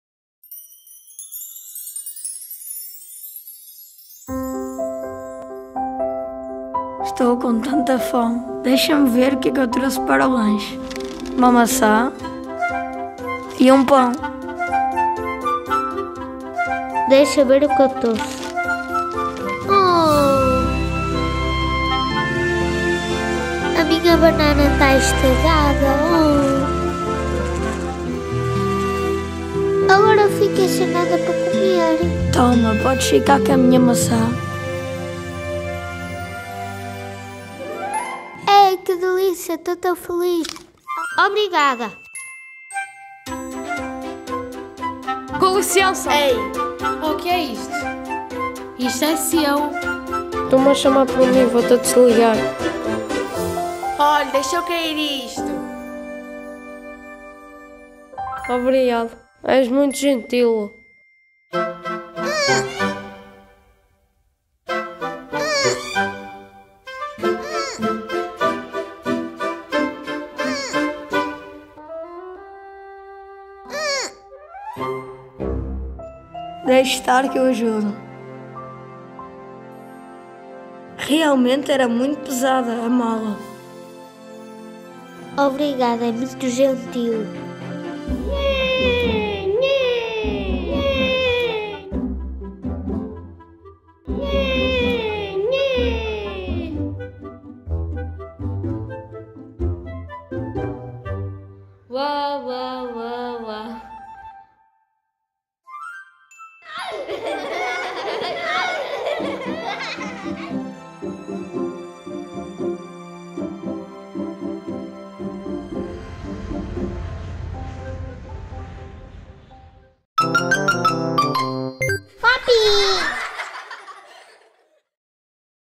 Colorir o Mundo com Bondade - CAA EB 2/3 Ramalho Ortigão
Dobragens de voz que dão vida aos personagens e a esta mensagem tão importante. Devemos ser sempre bons para o próximo e dar sempre o melhor de nós!

Vídeo produzido no Centro de Apoio à Aprendizagem da Escola Básica Ramalho Ortigão.

Vozes: Inês, Nelson, Luís, Ricardo, Tiago.

HAPI - Habilidades Artísticas Para Incluir
Nuno Teixeira
2021